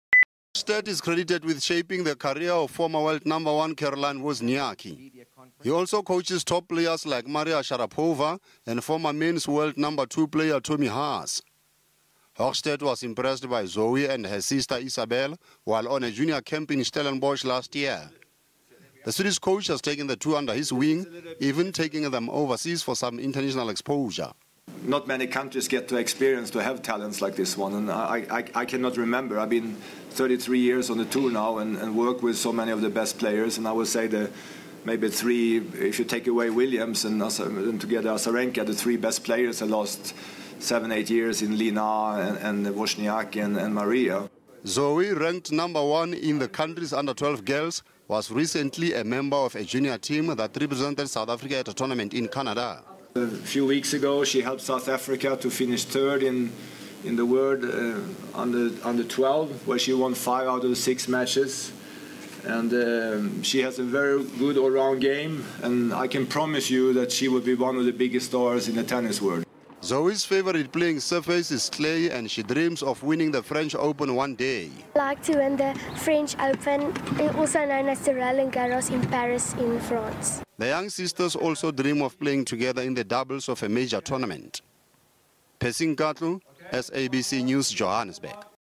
SA grooms future grand slam champ
South Africa has a future female grand slam champion and possibly a World number one tennis player in the making.

Top Swedish professional tennis coach, Thomas Högsted, who coaches some of the top tennis players in the world believes 11-year -old Zoe Kruger from Pretoria is a rare talent and will one day win the country an Olympic medal.